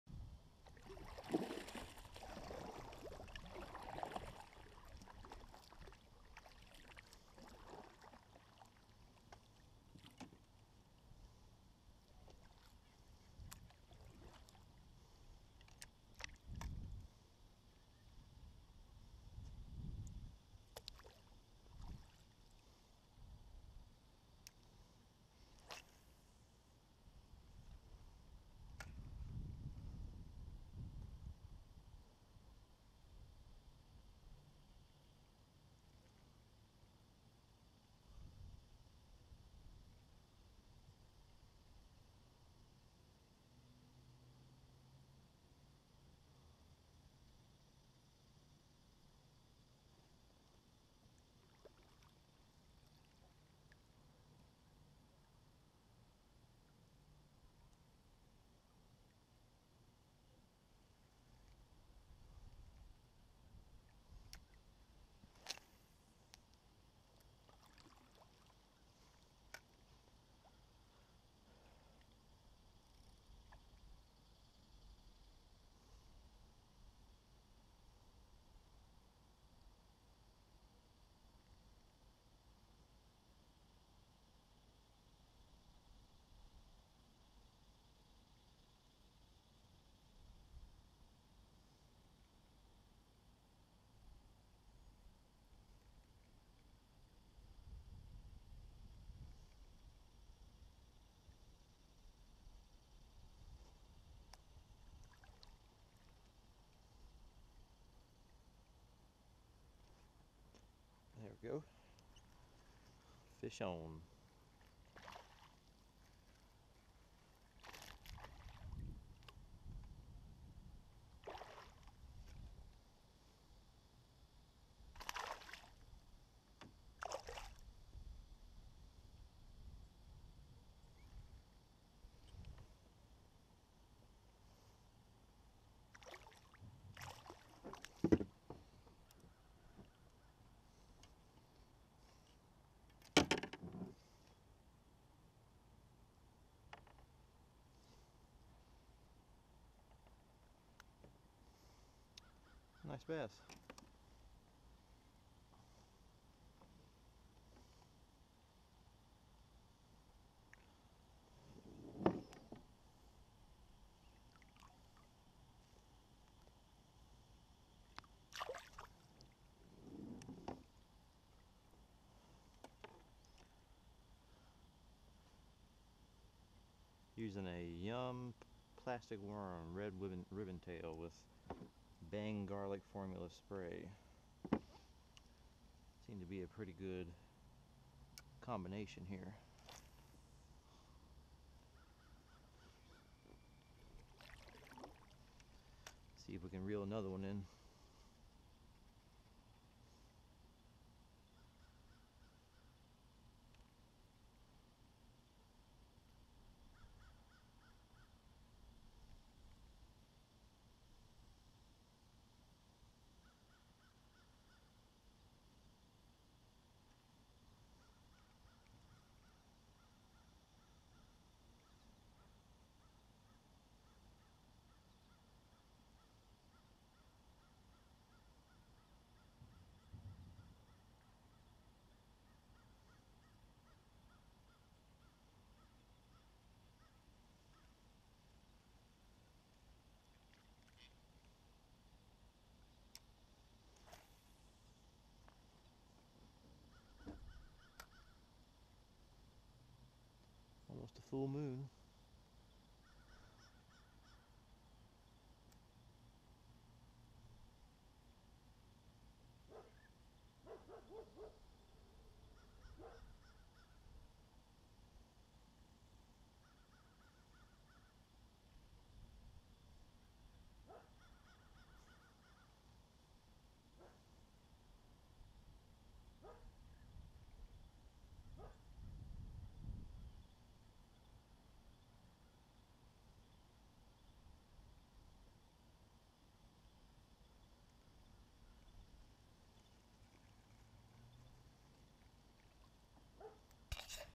Fishing from a Kayak with a Full Moon in Daytime
Doing a little fishing and kayaking during a full moon on September 15th 2013.